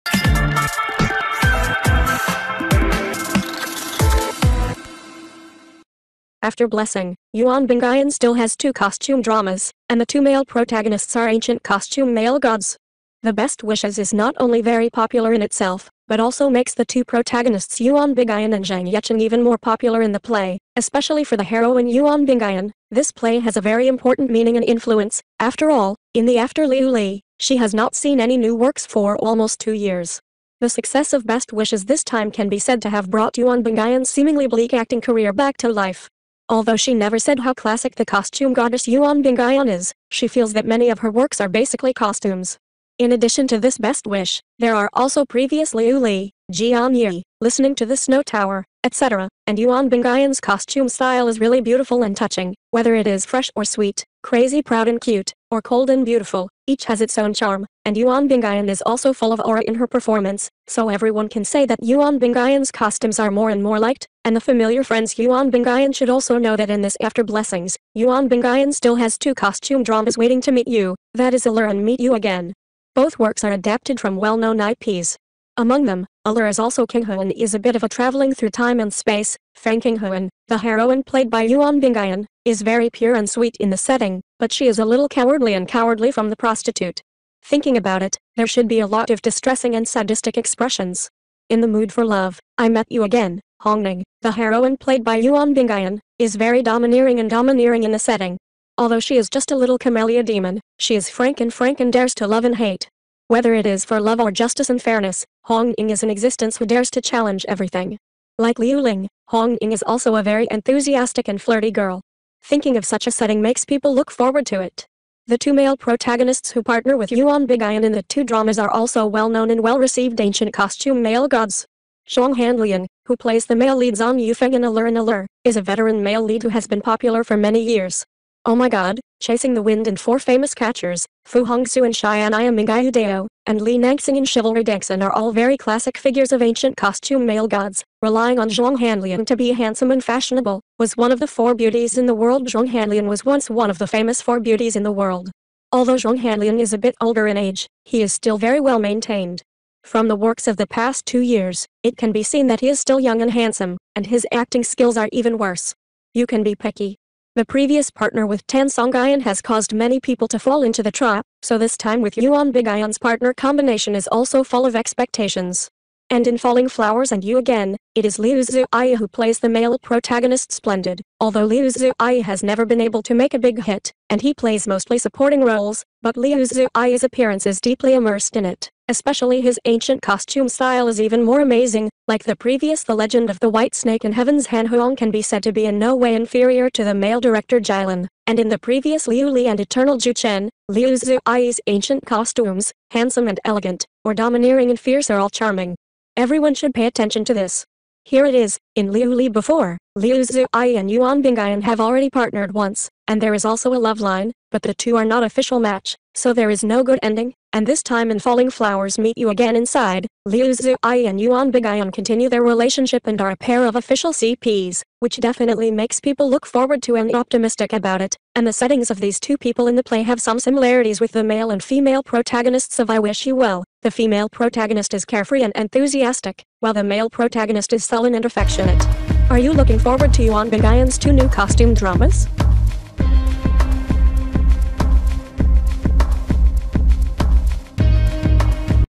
After "Blessing", Yuan Bingyan still has two costume dramas, and the two male protagonists are .....
《祝卿好》之后，袁冰妍还有两部古装剧，两位男主都是古装男神 | Daily Star News